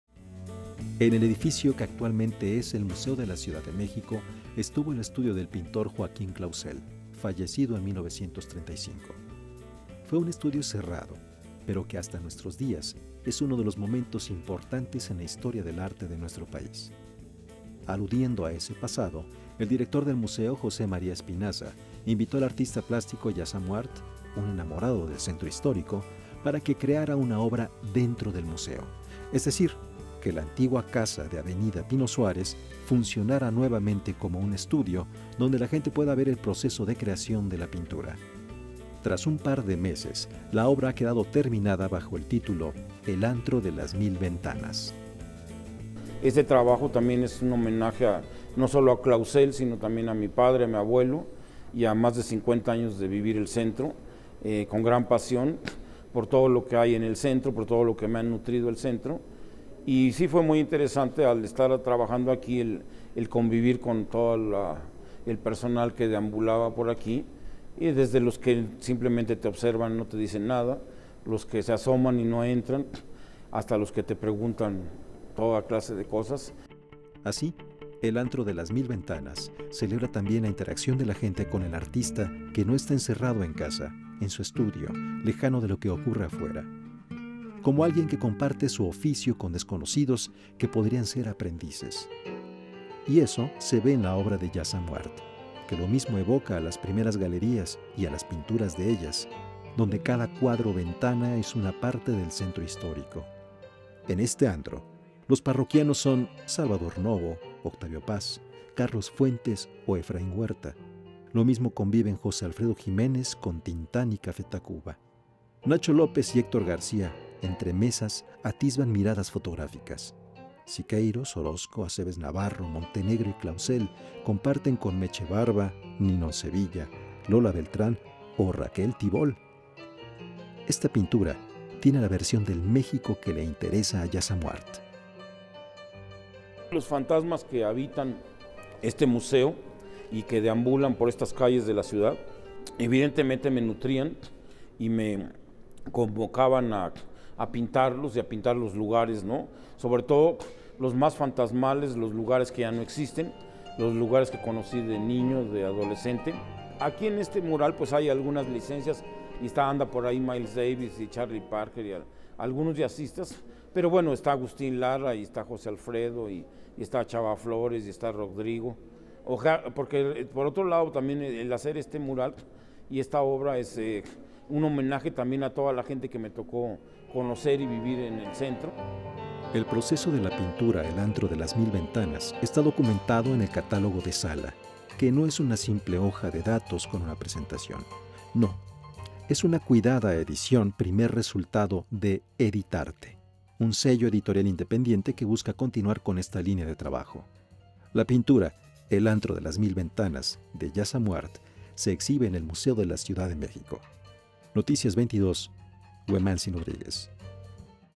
"El antro de las mil ventanas" de Jazzamoart
Después de un proceso de aproximadamente dos meses de creación, el artista Javier Vázquez Estupiñan, "Jazzamoart" exhibe el mural "El antro de las mil ventanas" en el Museo de la Ciudad de México.
Por: Huemanzin Rodríguez.